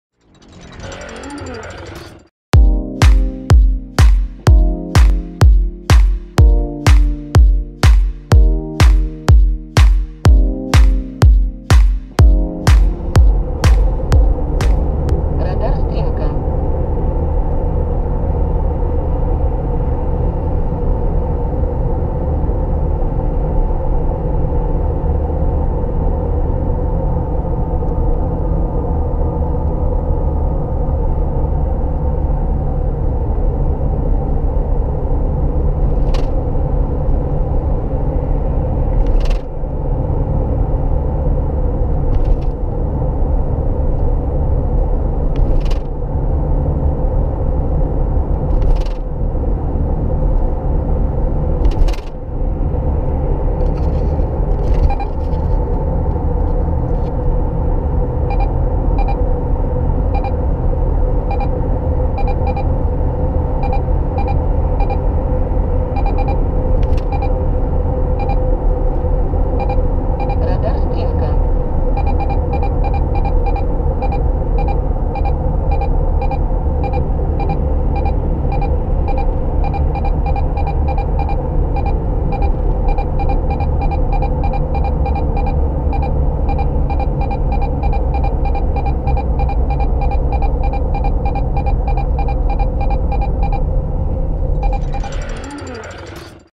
Neoline X COP 4300/Стрелка СТ
В Х-СОР 4300 встроен белый символьный Icon Text дисплей. Уникальность в отличной читаемости при любом освещении без потери информативности, при этом стоимость на уровне устройств с обычным светодиодным дисплеем.

Радар "Стрелка СТ" широко распространен в Москве и Мос. области. Данный радар может фиксировать как превышение скорости, так и проезд по полосе общественного транспорта.
Гарантированное обнаружение стрелок обсепечивается благодаря встроенному в X-COP 4300 модулю "Стрелка", а также актуальной базы GPS. Базы радаров обновляются каждые 2 недели и доступны на сайте